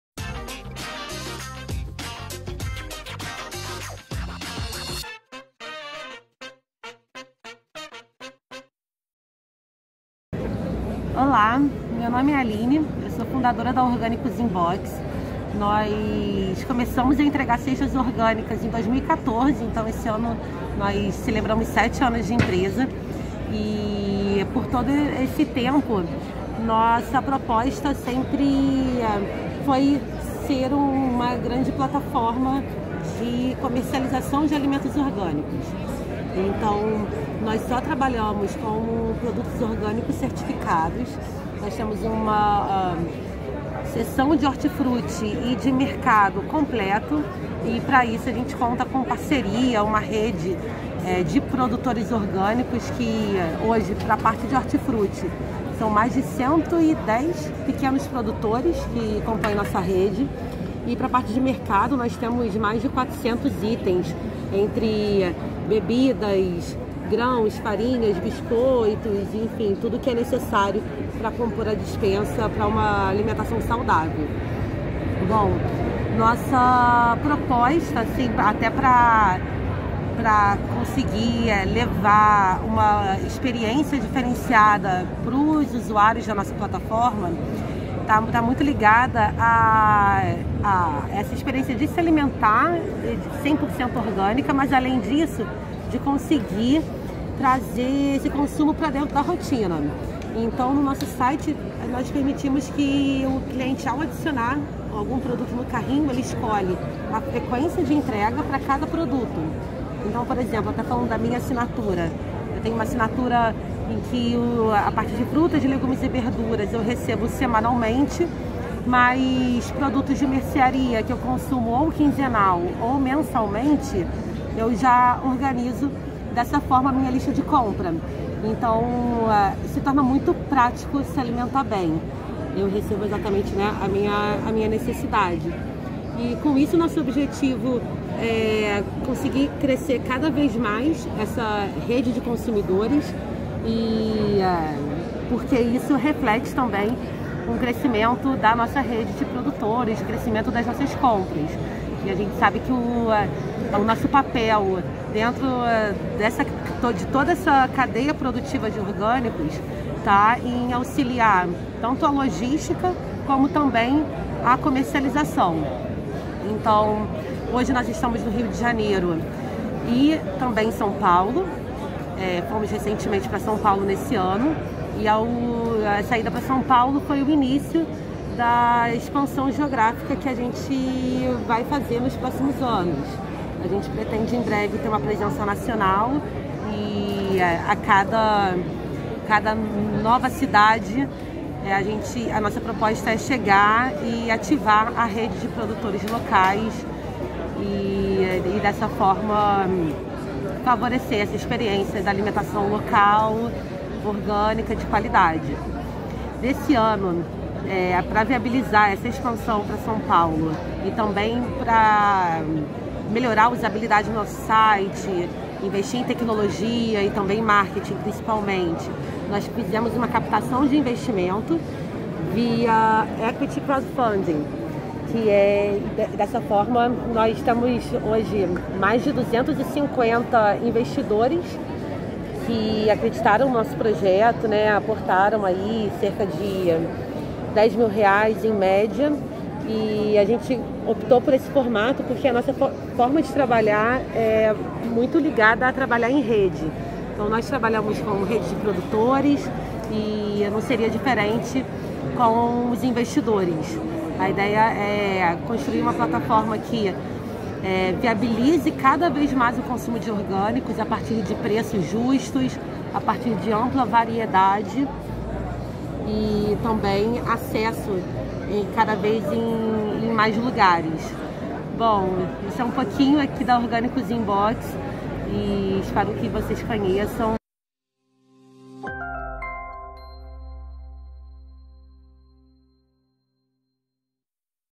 CI Orgânicos entrevista Aline Santolia, sócia fundadora do Orgânicos in Box
Aline Santolia, sócia fundadora do Orgânicos in Box, fala sobre como a empresa une praticidade e alimentação saudável para entregar a melhor experiência em produtos orgânicos, certificados, para seus consumidores. Assista ao vídeo e confira!

Nosso Instagram tem novas atualizações todos os dias sobre os orgânicos, siga-nos para não perder